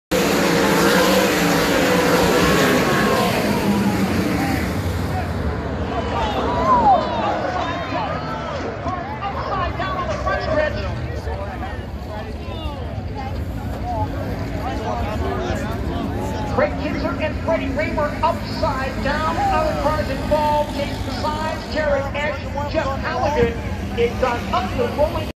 World of Outlaws Nittany Showdown Night 1 LCQ crash @ Port Royal Speedway 10-25-2019
Kinser, Rahmer, Sides, Esh crash